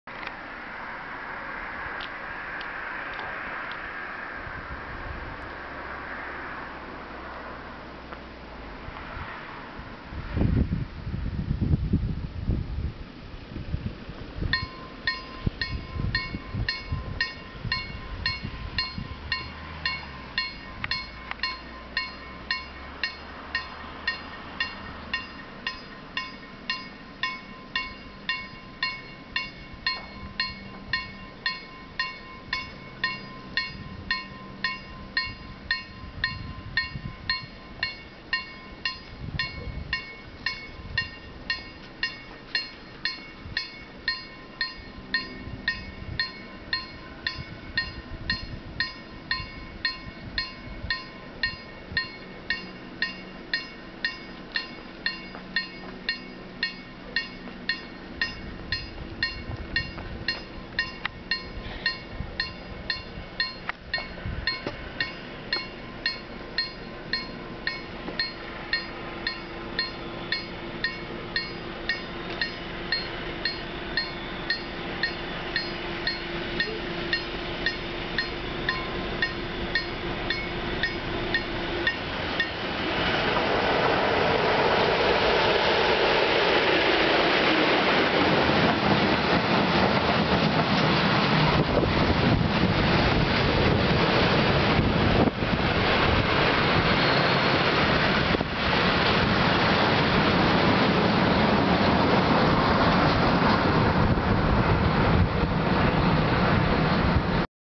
SM42 - 605 Gruszki Janówek 12-08-2009
Stonka CARGO z gruszkami przejeżdża przez p.o. Janówek